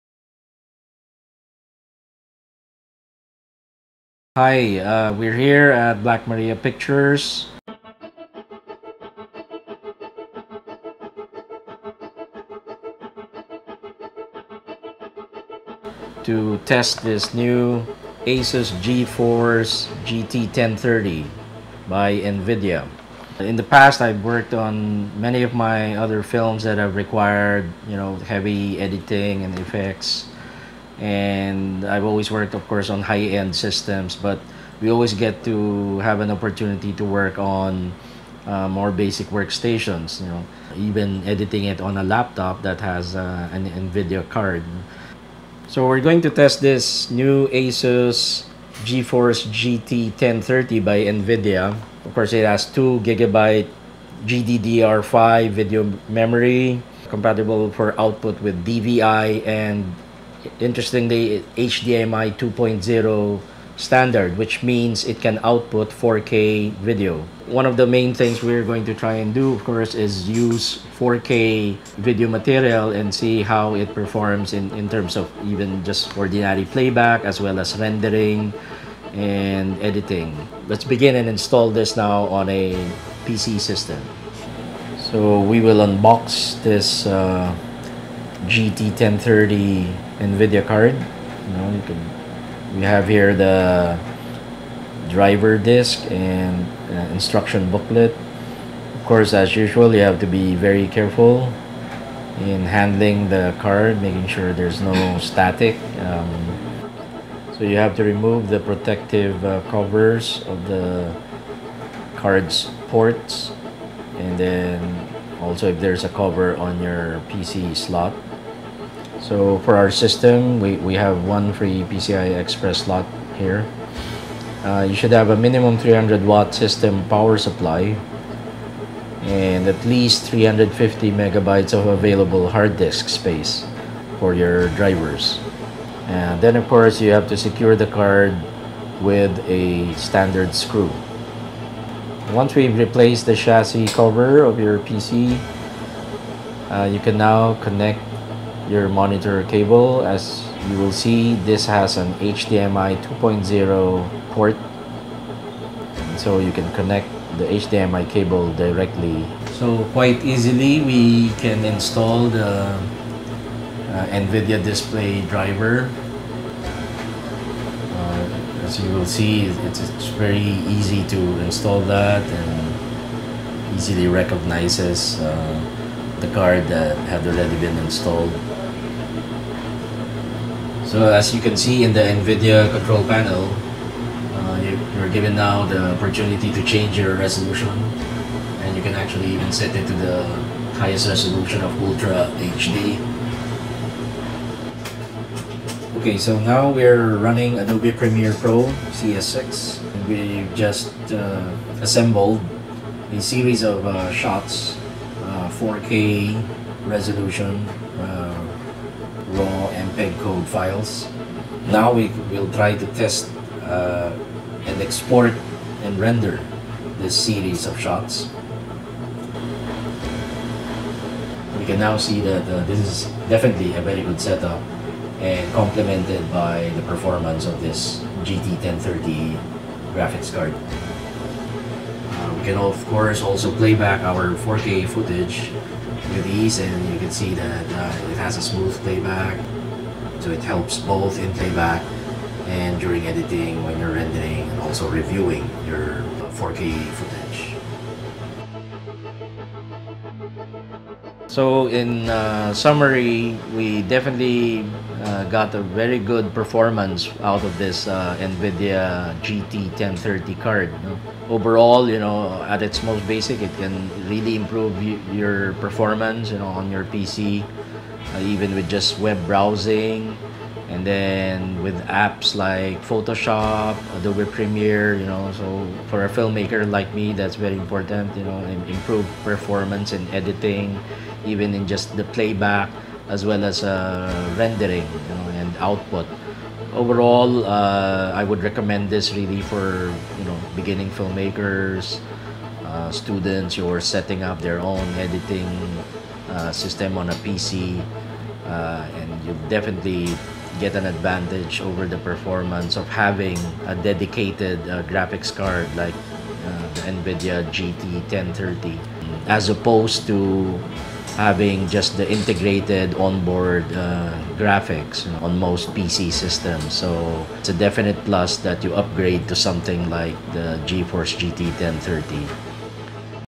Testing the ASUS NVIDIA GT1030 / Raymond Red at Black Maria
Testing the ASUS NVIDIA GEFORCE GT 1030 video graphics card on a standard PC set-up for video editing with 4K capability. Raymond Red tests the card with the help of Black Maria Pictures / Studios